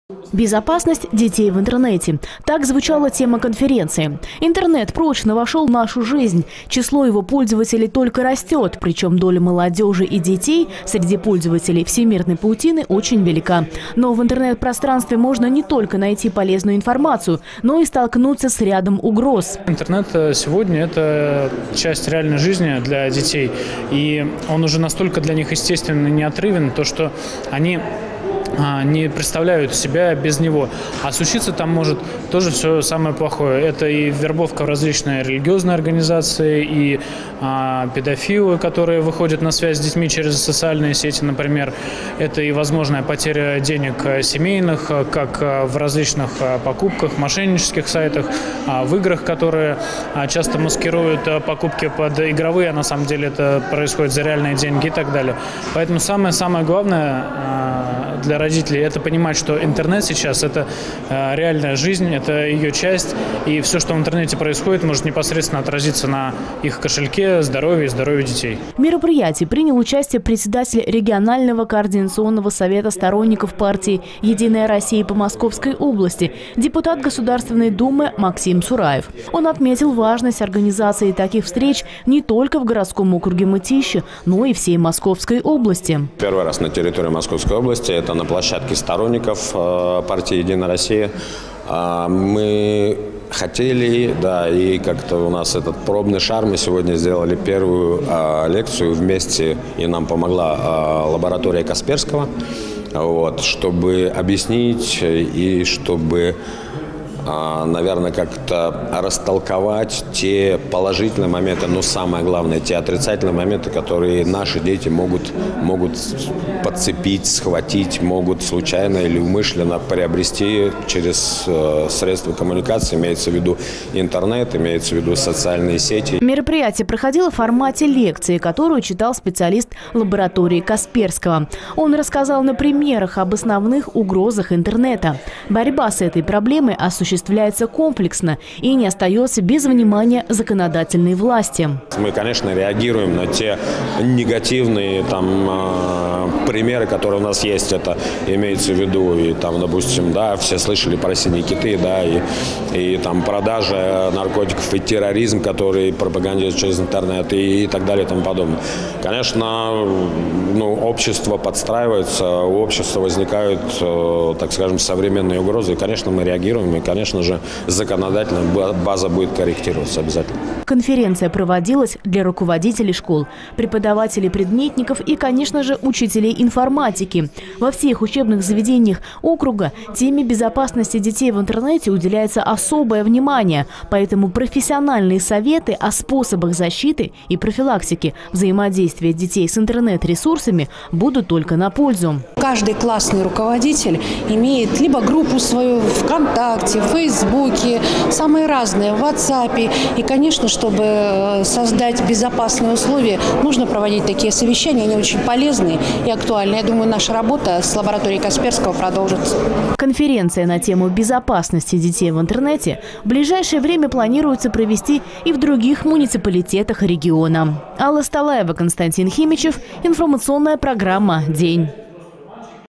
Проект «Безопасность детей в интернете»
Вопросы безопасности детей в интернет-пространстве обсуждали участники педагогической конференции в гимназии № 33. Тема эта актуальна и для детей, и для родителей, и для учителей. Выработать механизмы защиты — вот общая задача. Свое видение решения этой проблемы высказал депутат Государственной Думы Максим Сураев, который вместе со сторонниками партии «Единая Россия» принял участие в этом мероприятии.